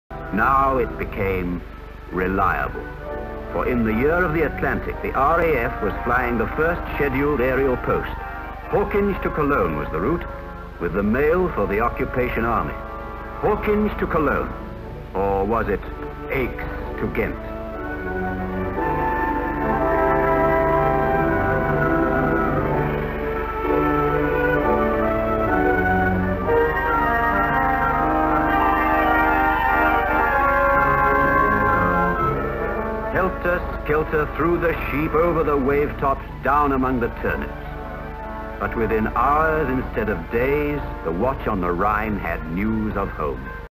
RAF Airco DH.9 Hawkinge-Cologne 1919 aerial postal service
The Airco DH.9, also known after 1920 as the de Havilland DH.9, was a British single-engined biplane bomber developed and deployed during the First World War.
video by Shell International Limited via the National Aerospace Library